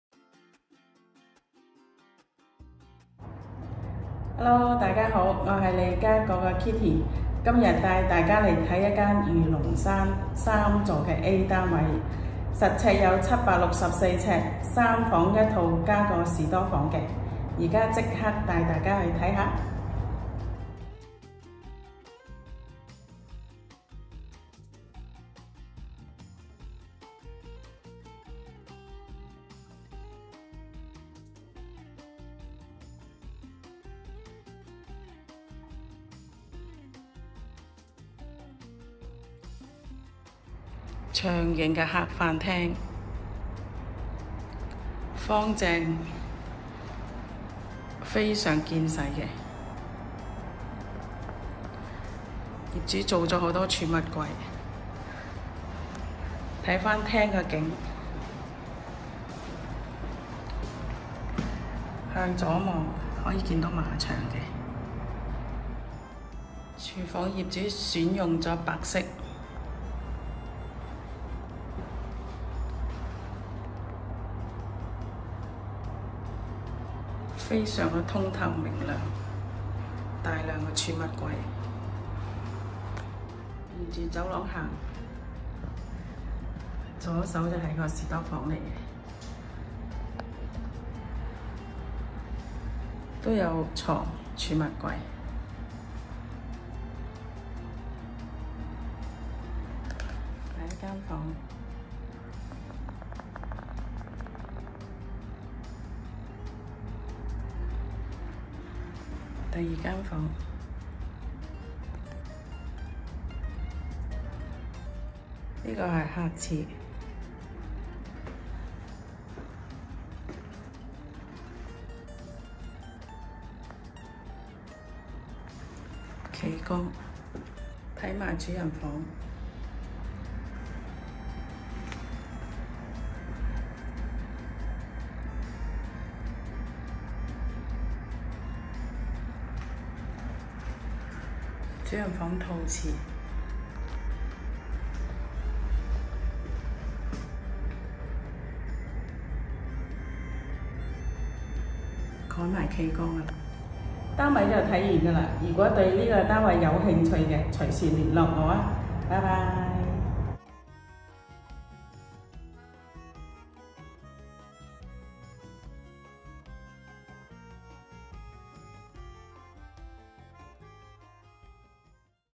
【KOL睇真盤】 Kitty Lo 筍盤推介：御龍山 3座 低層 A室
KOL睇真盤 – Kitty Lo 筍盤推介：
沙田  御龍山
物業地址：御龍山 3座 低層 A室（物業編號: CW06258287）
實用面積：764 呎
間隔 ： 3房1套+儲物室
賣點 ： 有匙即睇